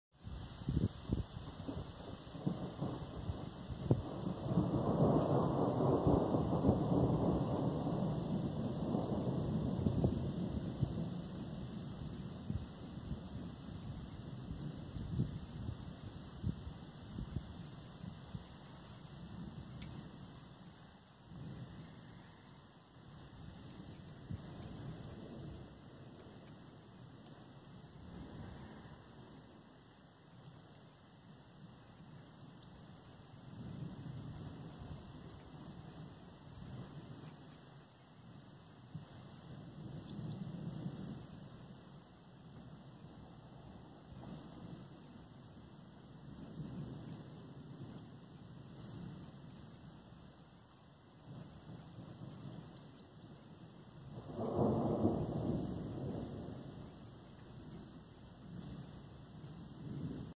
Ambient: Light Distant Thunder
A light, distant thunder in the countryside.

If you wish to use this clip in your own work feel free to do so, but a link back to this video would be greatly appreciated.